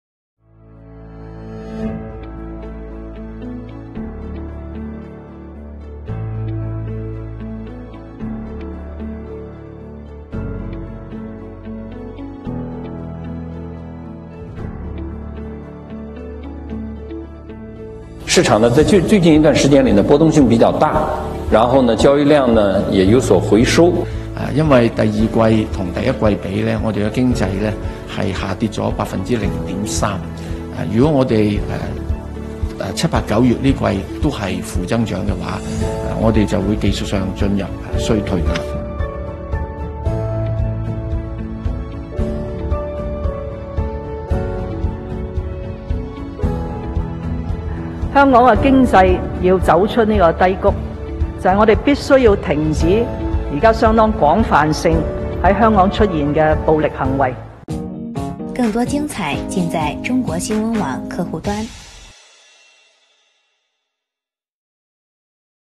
暴力示威冲击香港国际金融中心形象 经济衰退风险上升
从6月9日开始，香港激进示威者的暴力行为不断升级，已经影响了香港金融业。